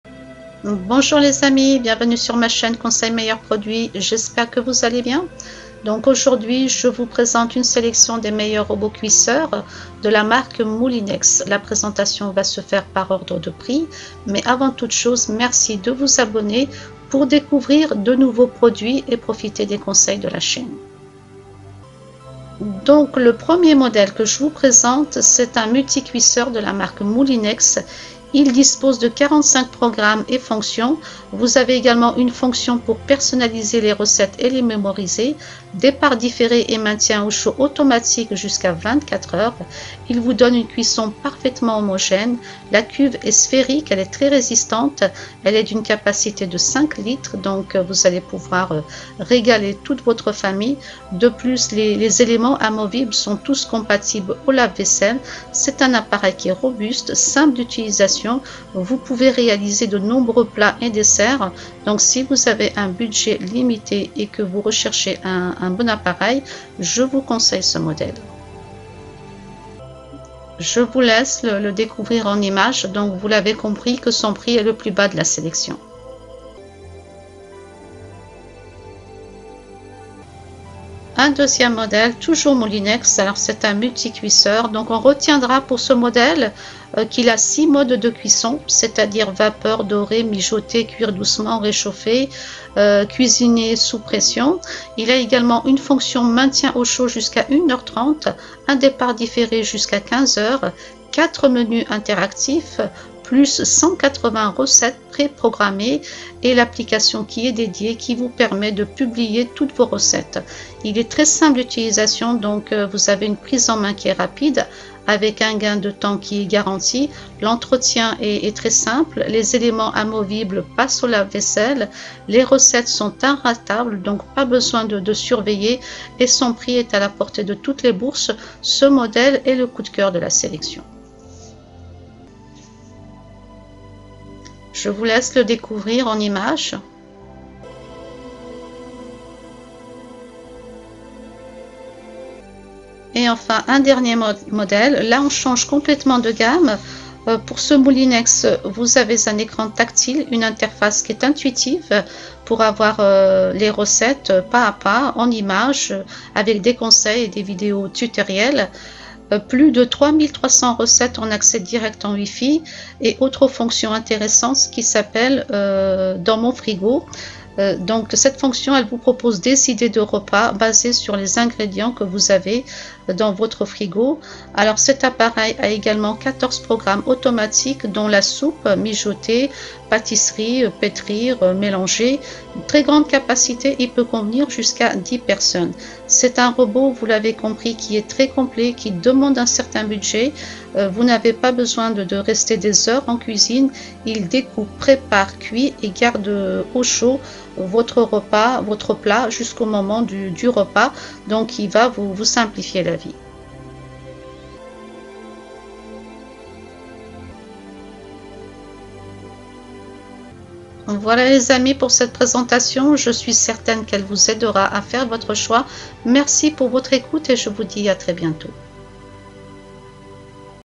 TOP 3 MEILLEURS ROBOTS CUISEURS 2021 MOULINEX
TOP 3 MEILLEURS ROBOTS CUISEURS  MOULINEX

Pour plus de détails ( prix et caractéristiques ) cliquez sur le lien du produit de votre choix

1 ROBOT MULTICUISEUR   MOULINEX  (prix le + bas de la sélection )

2 ROBOT MULTICUISEUR MOULINEX  ( coup de coeur )
3 ROBOT MULTICUISEUR MOULINEX (haut gamme )

 robot multifonction  pour réussir une infinité de recettes. Cuisinez à la maison sans effort , le robot cuiseur vous accompagne dans la réalisation de vos recettes quotidiennes.

Merci pour votre écoute et à bientôt

Images des marques
Musique libre des droits d'auteur ( merci chaîne papy nounn )